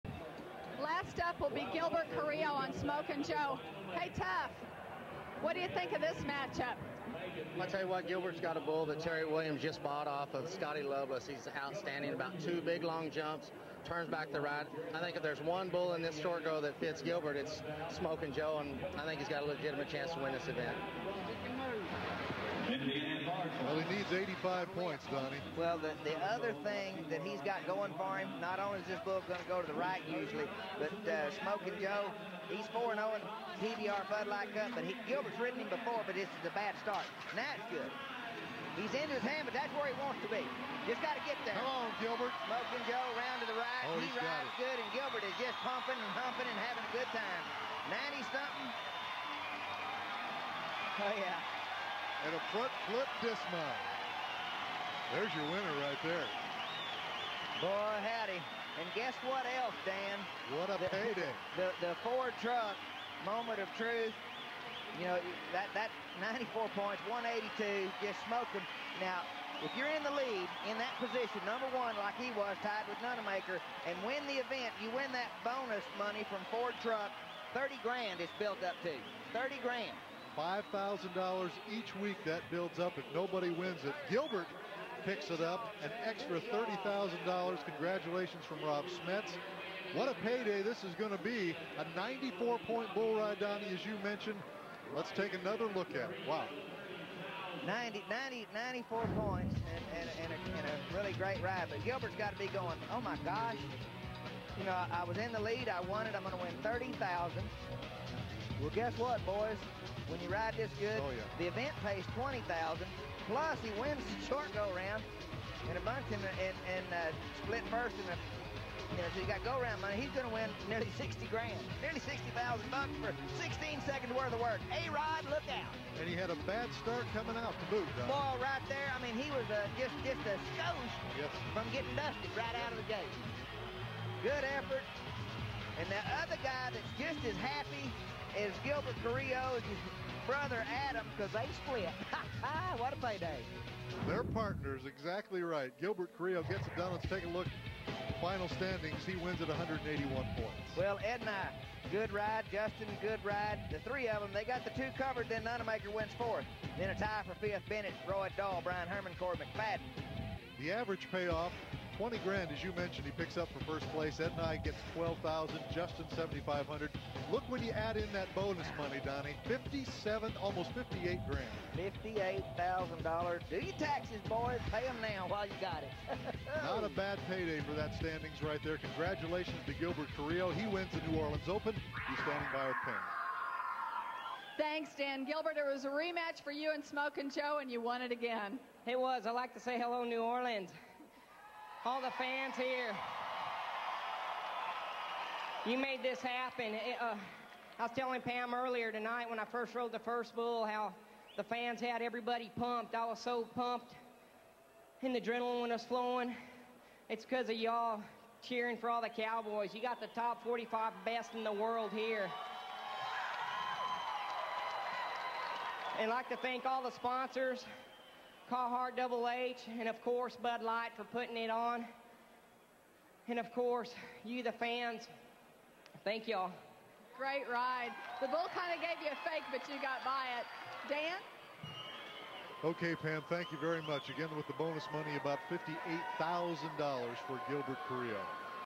PBR 2001: Gilbert Carrillo's Enormous Payday on Smokin Joe (94 pts)
The 1994 BRO Champion, big brother Gilbert Carrillo masters Terry Williams & David Bailey's +2 Smokin' Joe during the Western Union Money Round to win the 2001 New Orleans Open in New Orleans, LA. Gilbert had quite the bag of moolah after the whole event. He split the first round win, then won the Short-go, which of course gave him the event title, and to top it off, he claimed the old Ford Truck Moment of Truth bonus, which was a promotion reserved for the rider(s) who had the highest score(s) going into the Championship Round. If that rider could convert his lead in the preliminary round into an overall event championship, he would be awarded the bonus. If not, the bonus would go unclaimed and would be increased by $5,000 until it would get reclaimed and re-set. The bonus for this event for $30,000, so that combined with all the other winnings from the event title and round wins, Gilbert's overall paycheck was $57,895, making it one of the biggest paydays at a regular season PBR event. Bull score: 46.00